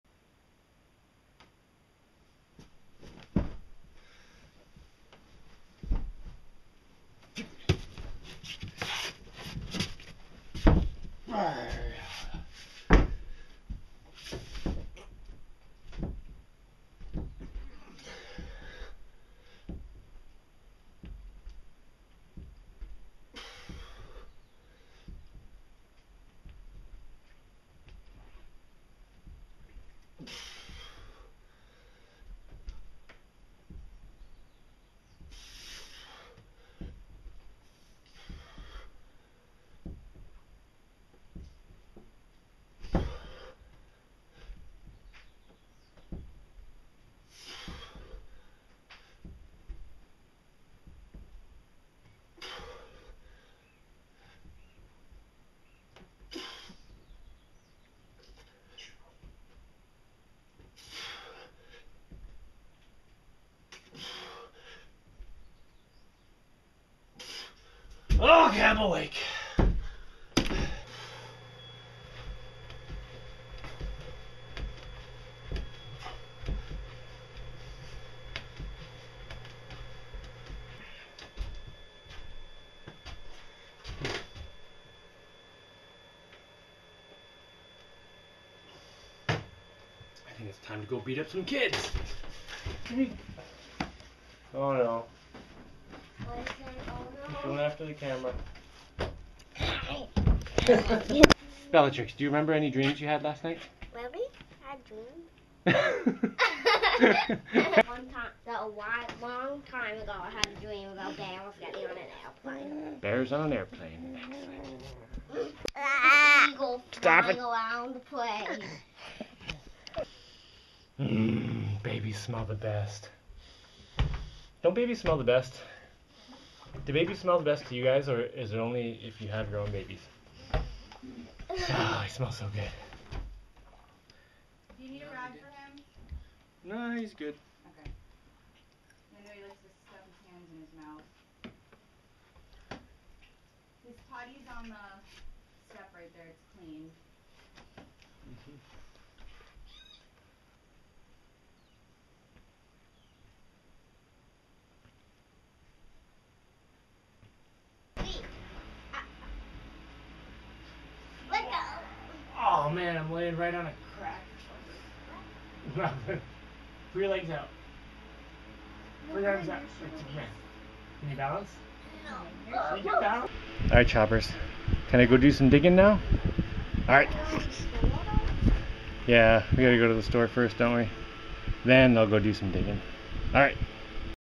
Better Than Coffee
This is an average morning.  Roughly 7-9am-ish.  I also made coconut banana potato pancakes and did some other stuff with the kids before going out to do some heavy lifting.  :-)